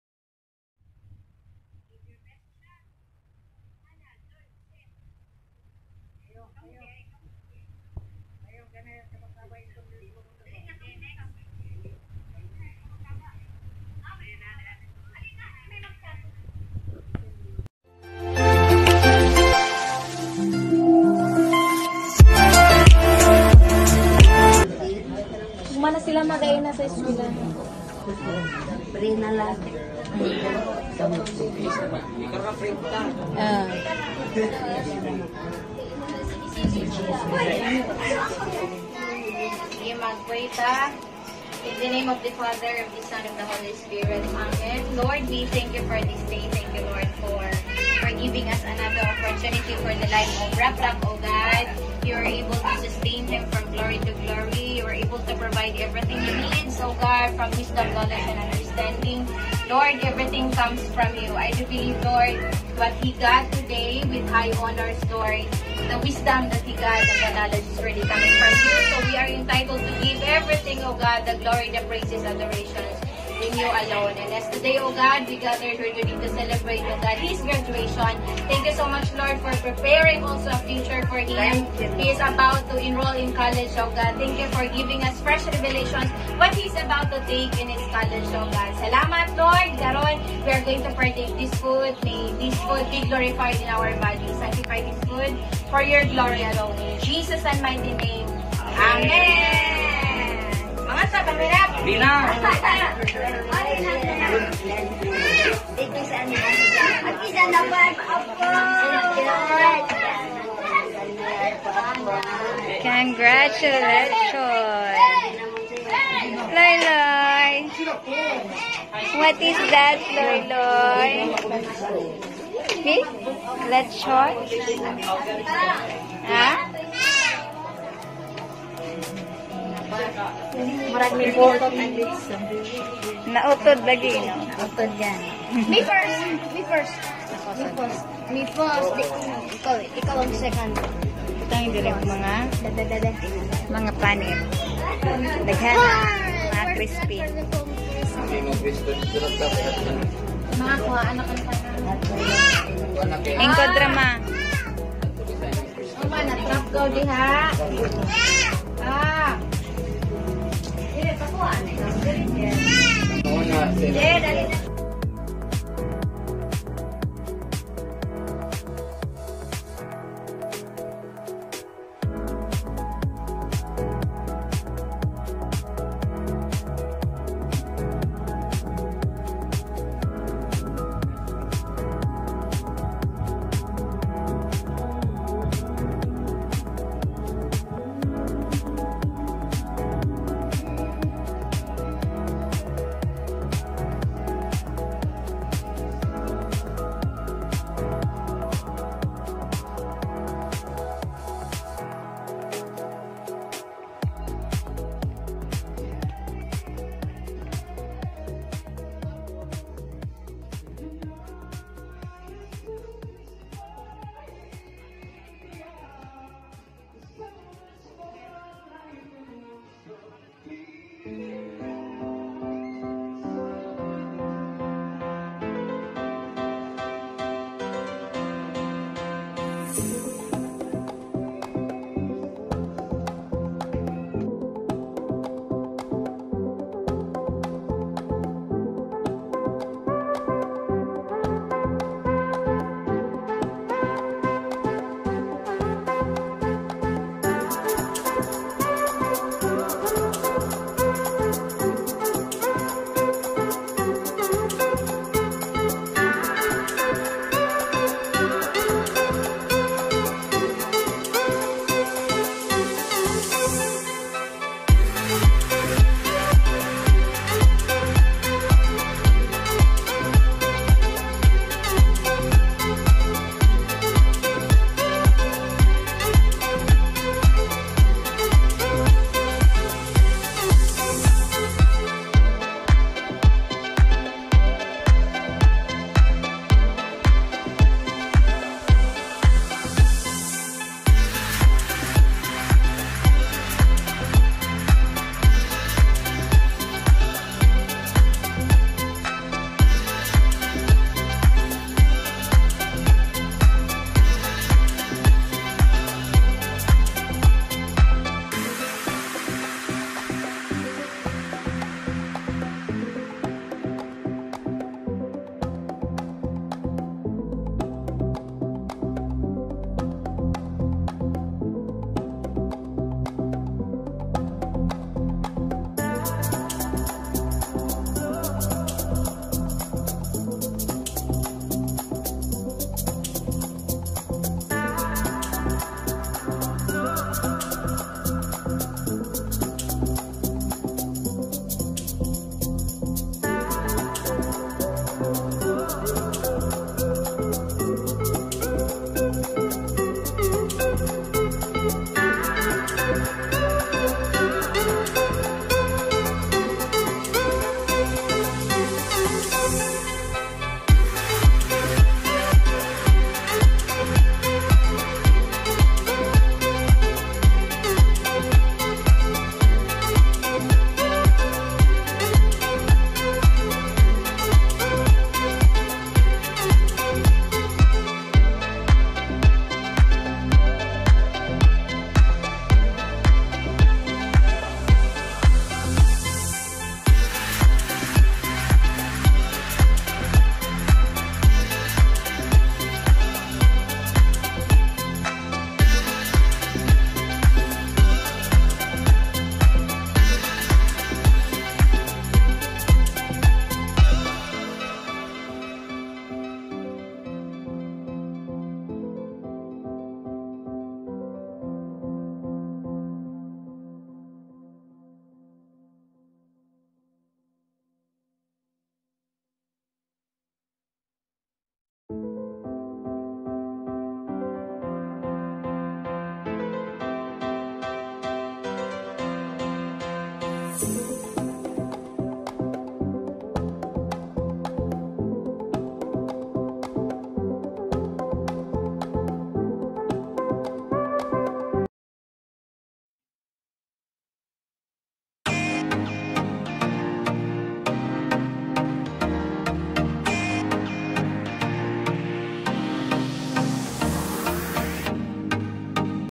THANKSGIVING CELEBRATION II RAPHY G. GUMAPAC II With High Honors II Part 2
This is a simple thanksgiving celebration of our nephew for his achievement as high honors in high school. Keep it up pamangkin and continue to aim higher. God bless!